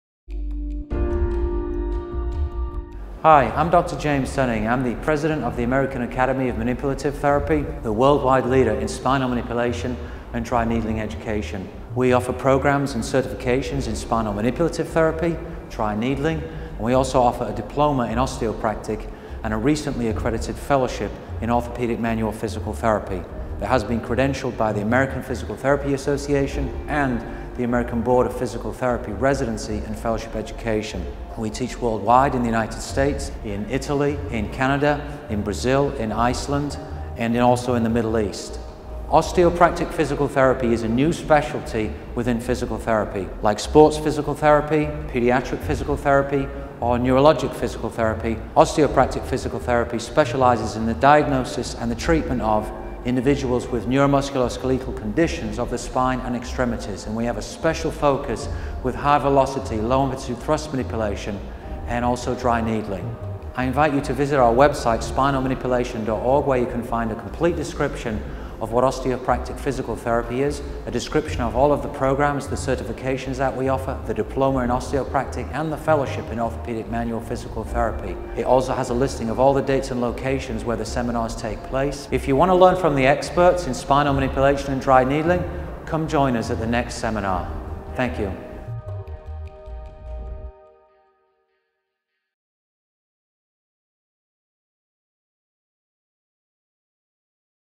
Osteopractor - The Neuromusculoskeletal Specialists | AAMT Fellowship in OMPT
The American Academy of Manipulative Therapy (AAMT) offers Certification in Spinal Manipulative Therapy (Cert. SMT), Certification in Dry Needling (Cert. DN), the Diploma in Osteopractic, and a post-graduate APTA-accredited Fellowship in Orthopaedic Manual Physical Therapy for Physical Therapists